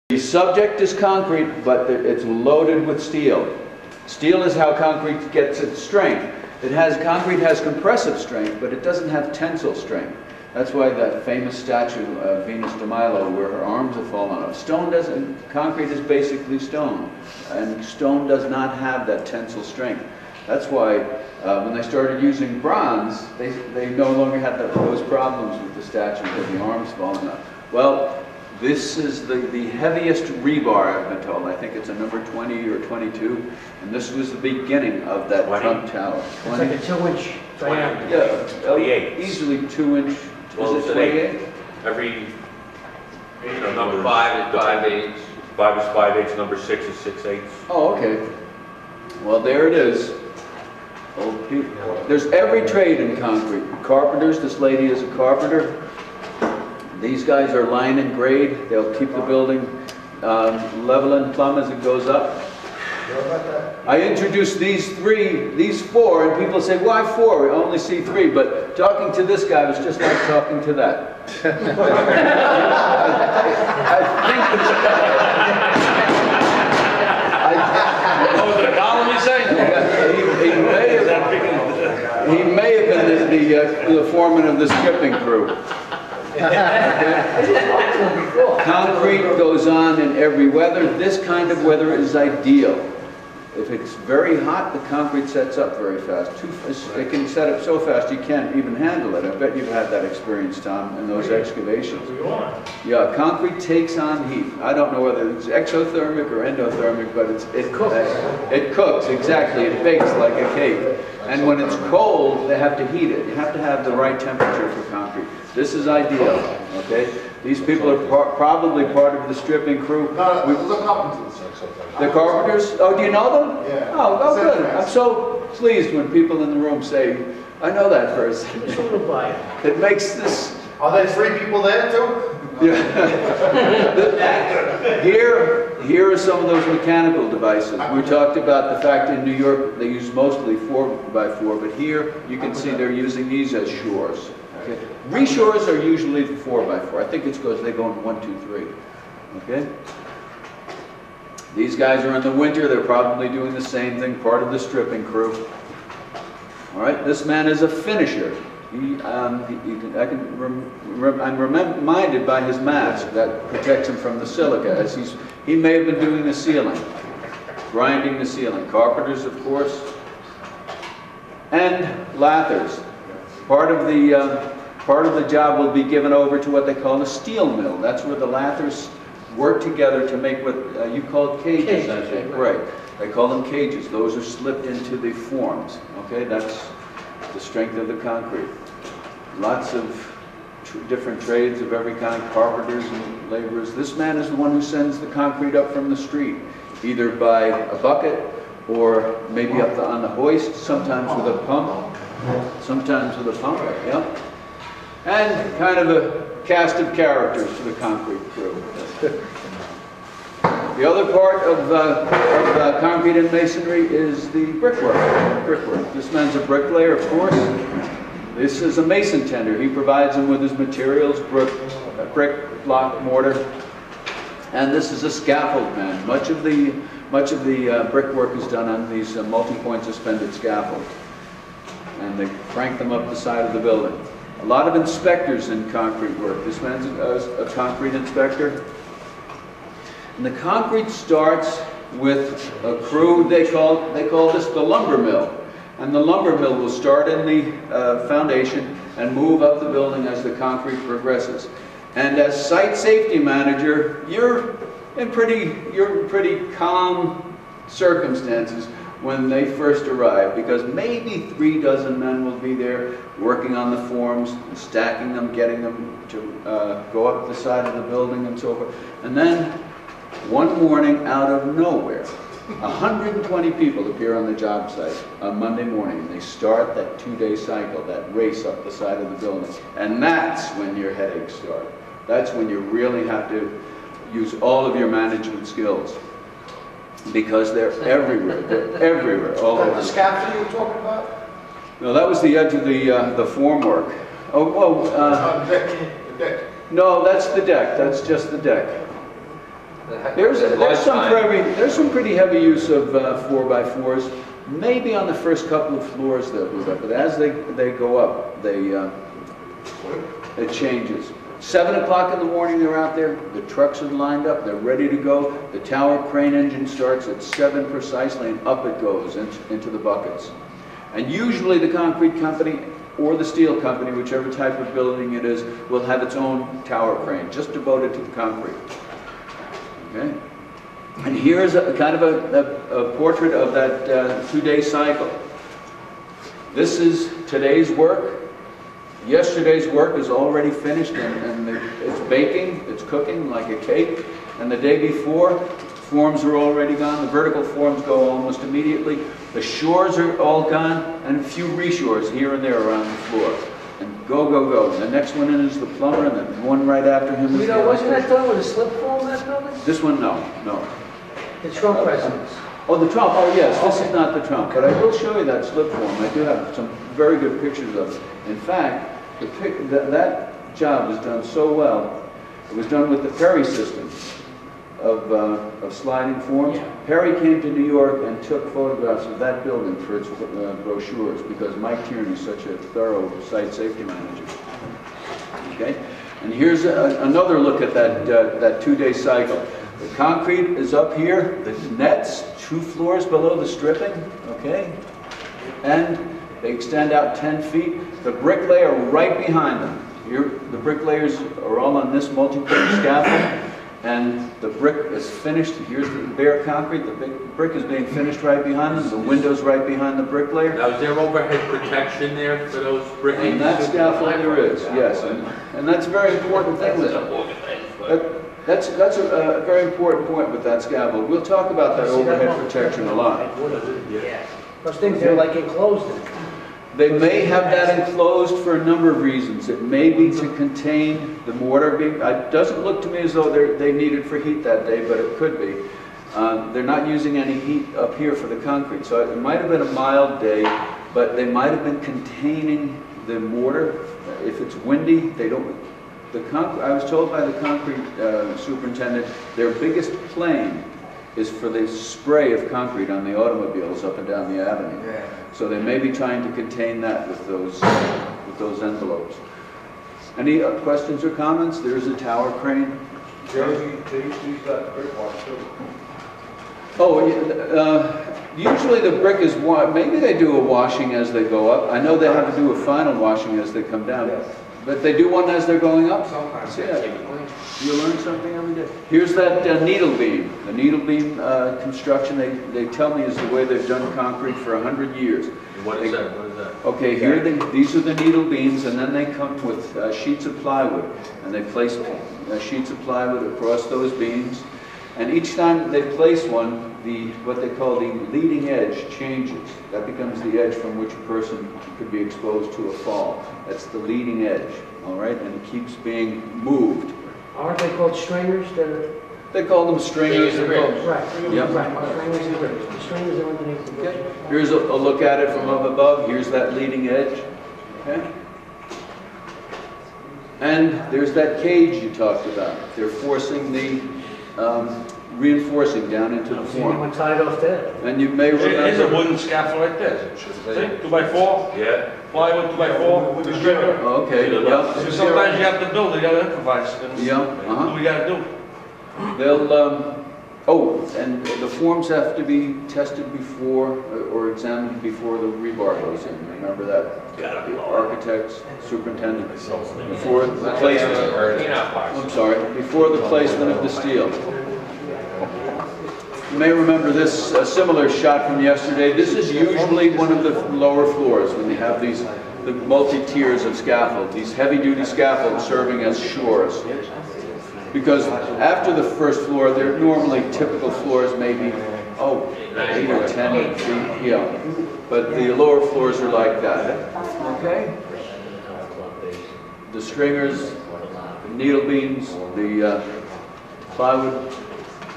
Concrete 10 Minutes
Guido presents a 10 minute slide presentation for Site Safety Managers on Concrete.